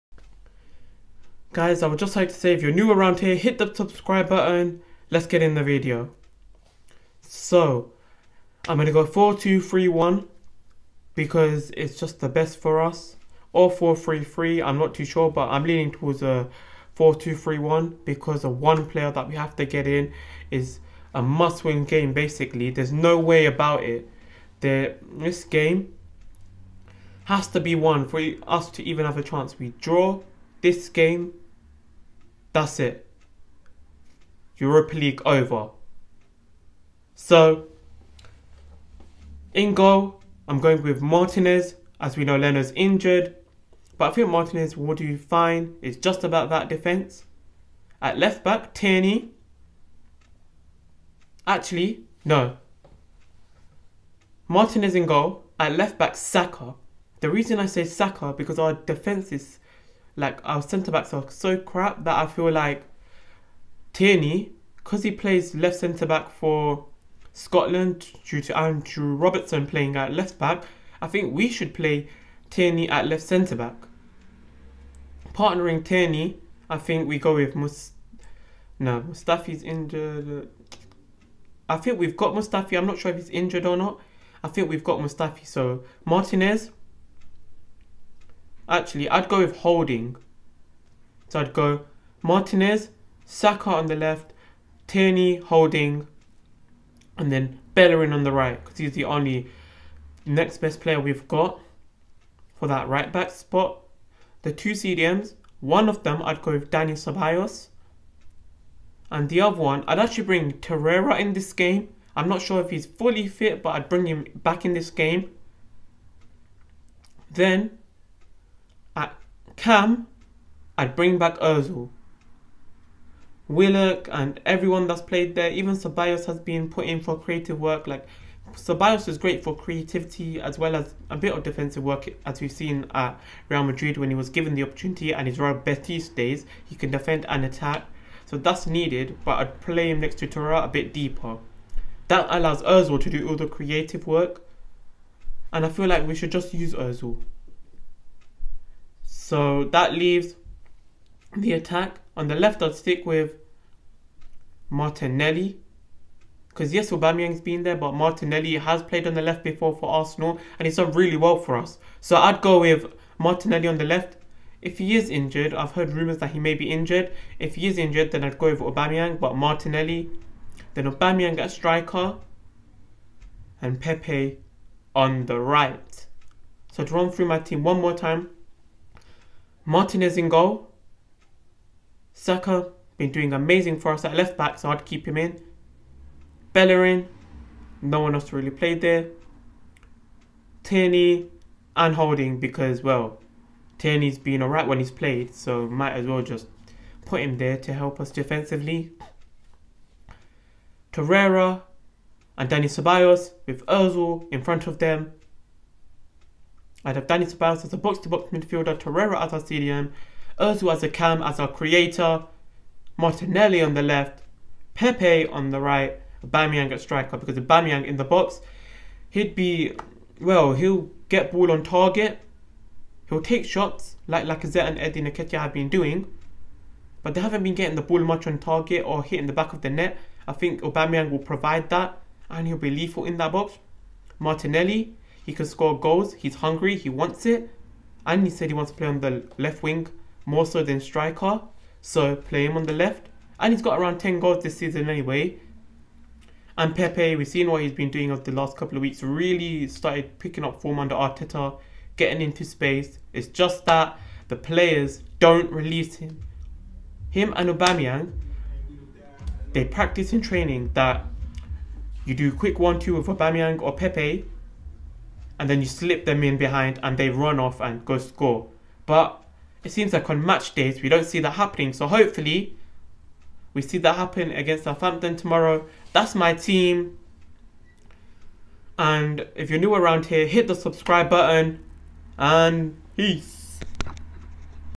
My line up vs Southampton
Hi xplosive army. Here is my lineup for the arsenal vs Southampton game. Found out Torreira isn’t fit and so is xhaka. Also martinelli is fit to play.